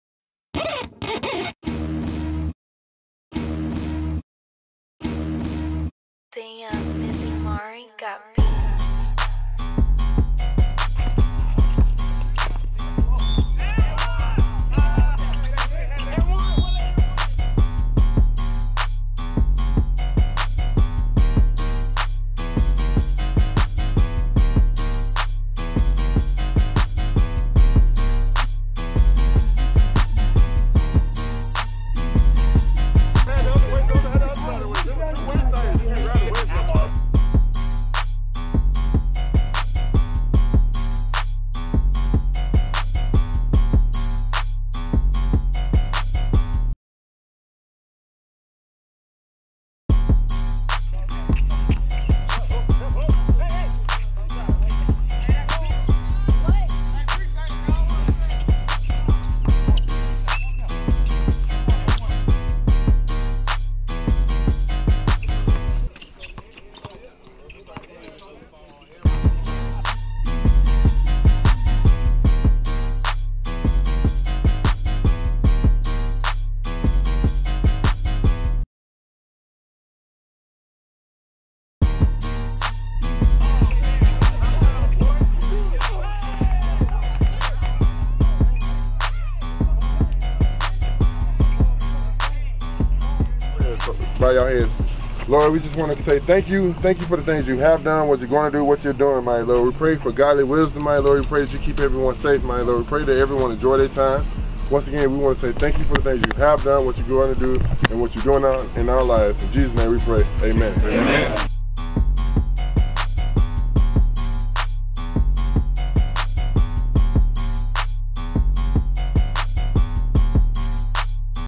Justin Houston Celebrity Weekend 1st Annual 3v3 Streetball Tournament
Statesboro GA June 24, 2017
$3,000 cash prize!

14 teams, 1 Champion, OMB!